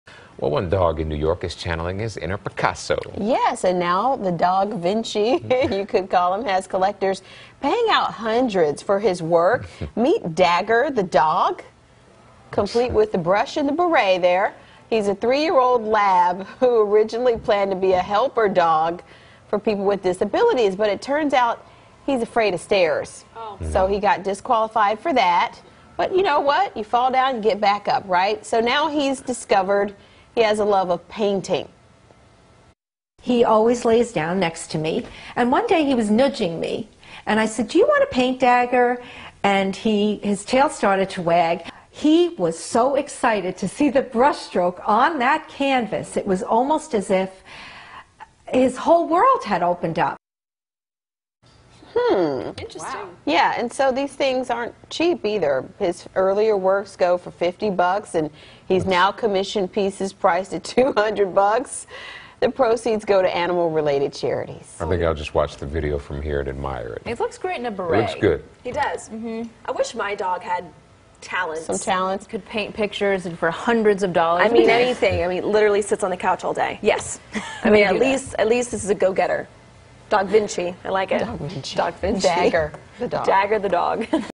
Painting dog has collectors paying hundreds for his work
A New York dog has art collectors paying hundreds for his work.